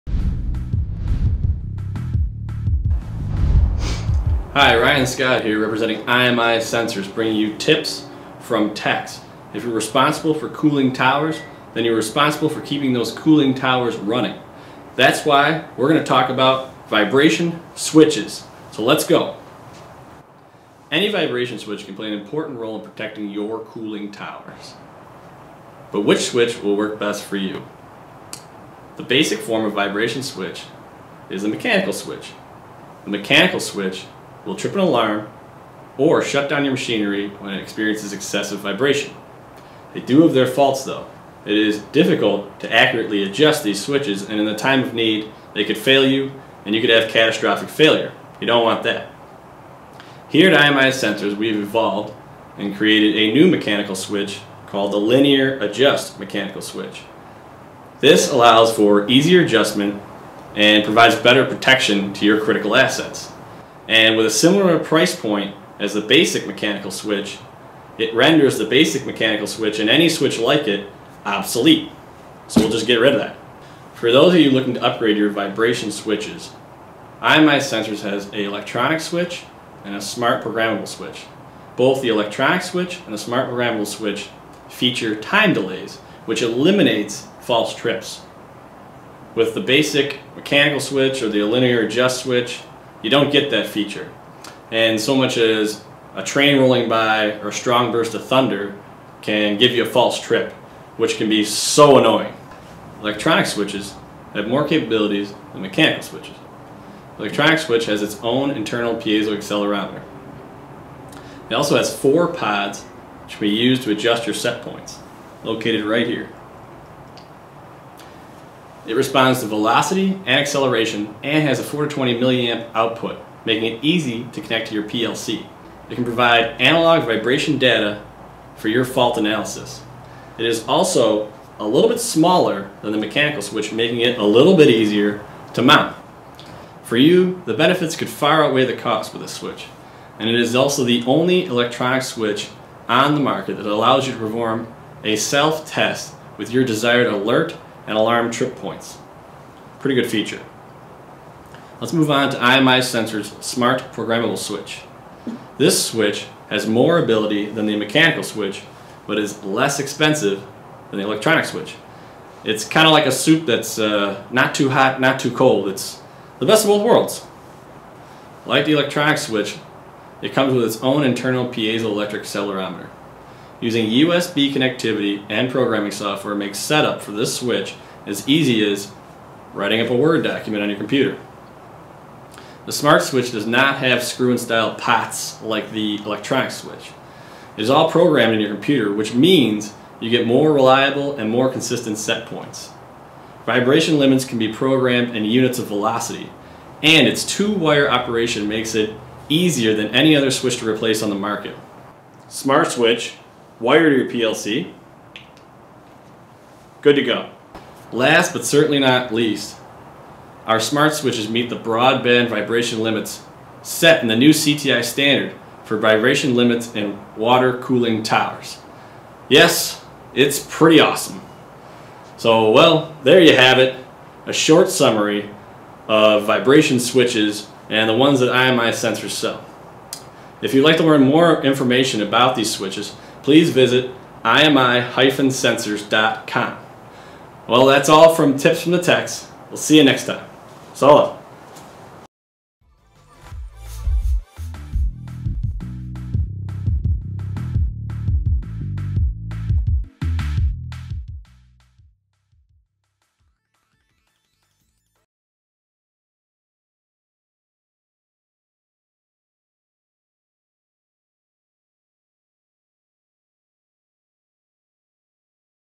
Vibration Switches for Cooling Towers - A Selection Guide
What is a Vibration Switch?

A vibration switch is a simple protection device that senses vibration and triggers an alarm or shuts down a machine if the vibration exceeds a preset threshold level. The vibration switch can sense vibration due to faults such as imbalance, misalignment, looseness, worn bearings, cracked gears or lack of lubrication. IMI Sensors offers a complete selection of electronic and mechanical vibration switches, including our INDUSTRY EXCLUSIVE "Smart" Vibration Switch, to suit many applications and installations. Check it Out!